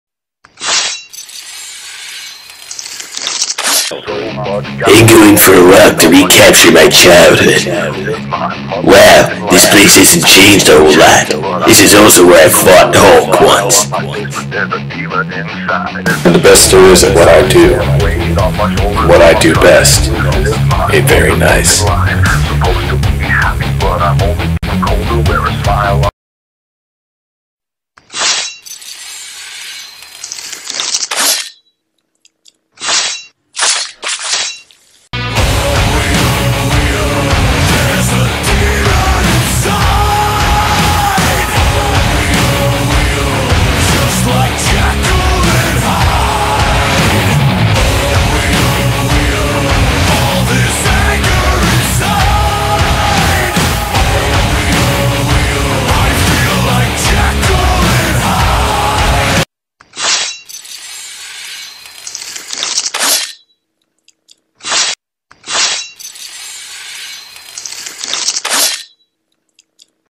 marvel chronicals audio drama episode 1 teaser treiler
This is a teaser for my  new fan marvel chronicles audio drama series similar to Karl Dotun's    but with a different continuity,
Wolverine, Cyclops and beast are taken but here is a script.